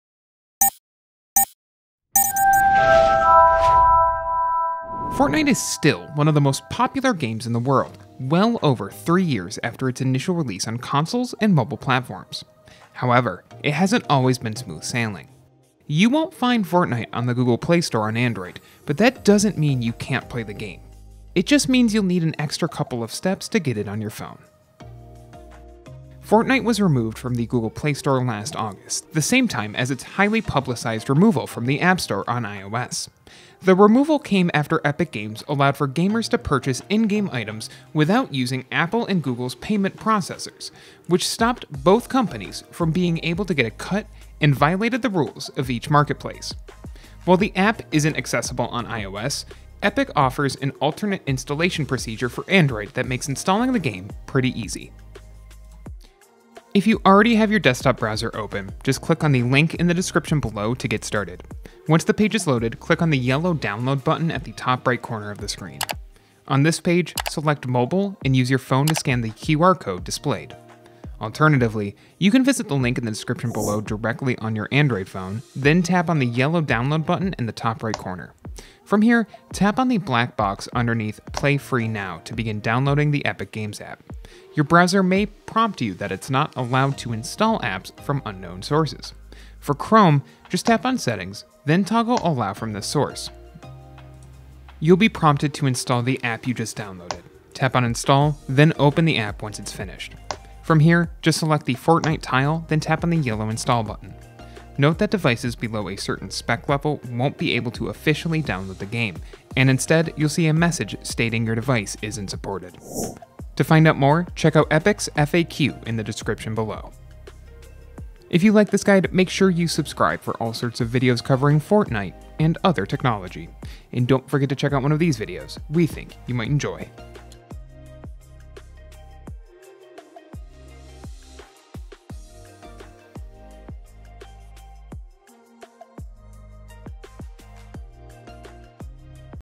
How to Download Fortnite on Android in 2021!
You won’t find Fortnite on the Google Play Store on Android, but that doesn’t mean you can’t play the game. It just means you’ll need a couple of extra steps to get it on your phone.